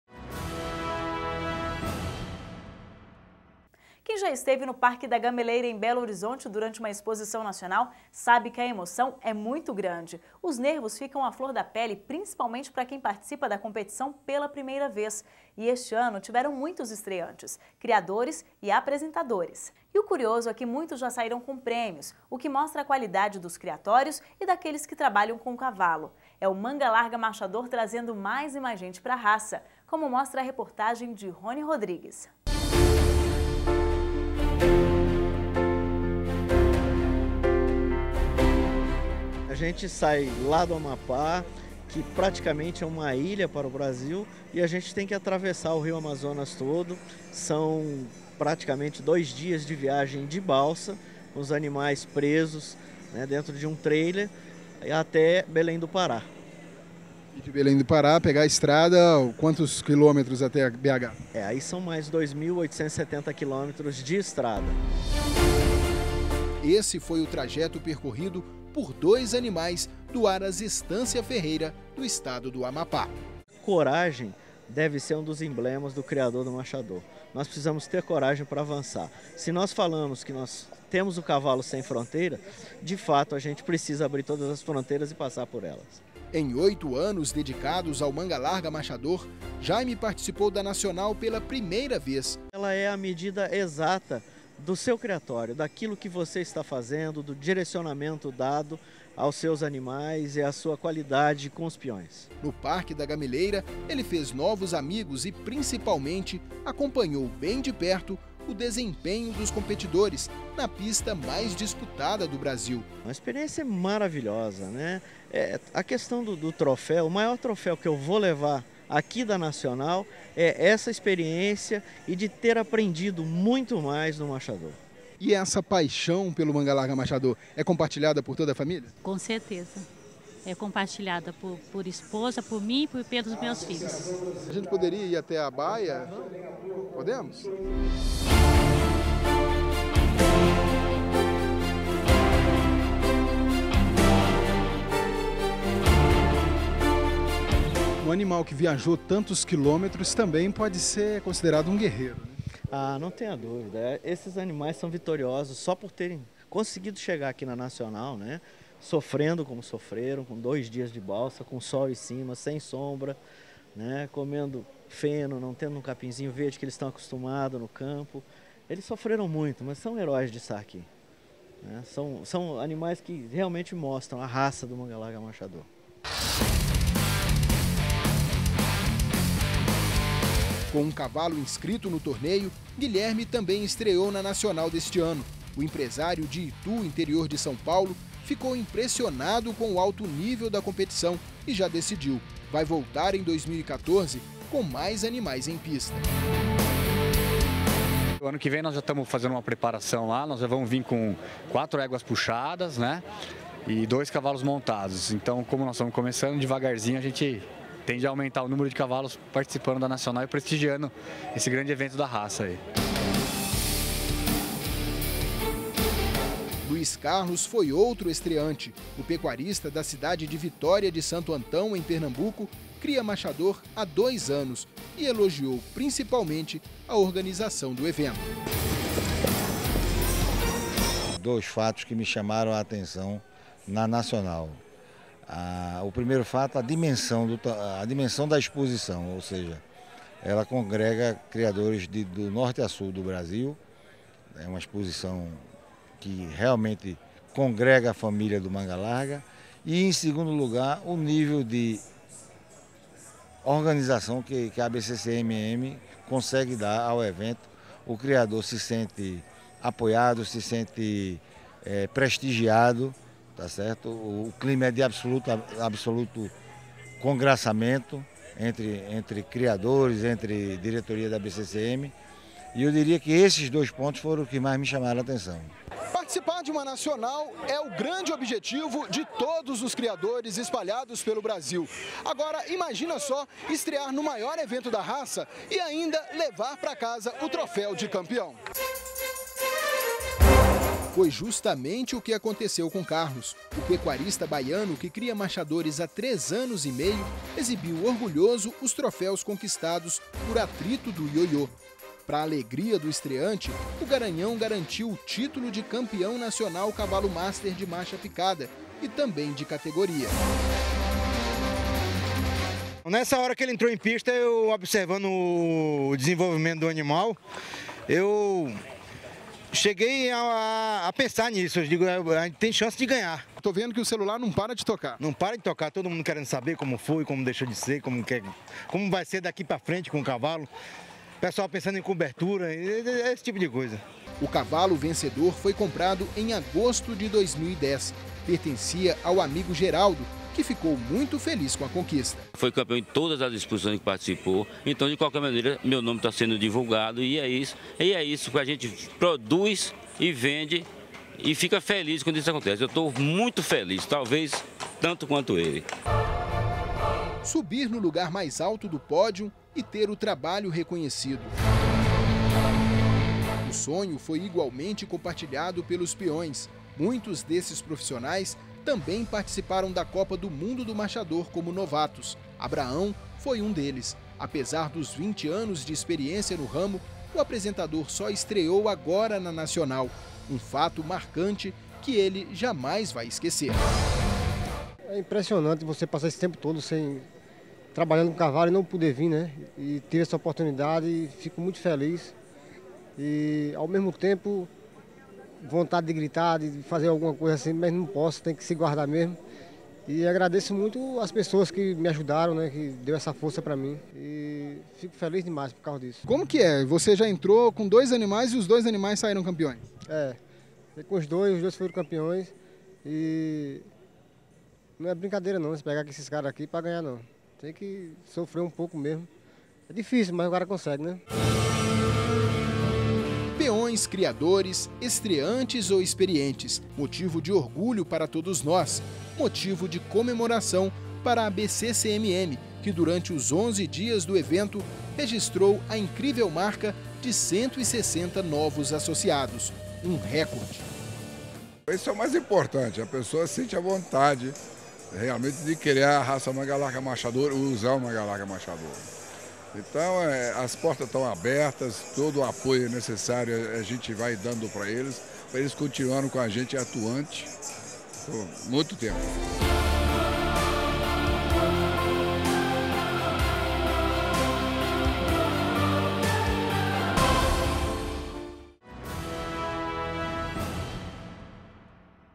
CRIADORES E PEÕES ESTREANTES - 32ª EXPOSIÇÃO NACIONAL
Quem já esteve no Parque da Gameleira, em Belo Horizonte, durante uma Exposição Nacional, sabe que a emoção é muito grande!.. Os nervos ficam à flor da pele, principalmente para quem participa da competição pela primeira vez... E este ano, teve muitos estreantes: criadores e apresentadores. E o curioso é que muitos já saíram com prêmios, o que mostra a qualidade dos criatórios e daqueles que trabalham com o cavalo./ É o Mangalarga Marchador trazendo mais e mais gente para a raça./ Como mostra a reportagem de Ronny Rodrigues.